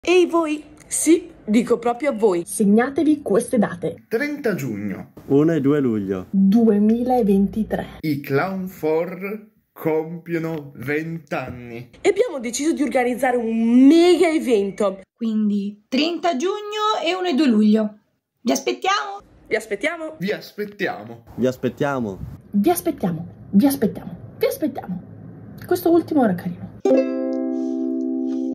EVENTO CLOWN4 - 30 Giugno, 1-2 Luglio 2023
Festeggiamo questi 20 anni in grande stile, con un festival! Concerti, spettacoli, workshop e molto altro a Oppeano -  "IL BENE FA BENE" 30 Giugno, 1-2 Luglio 2023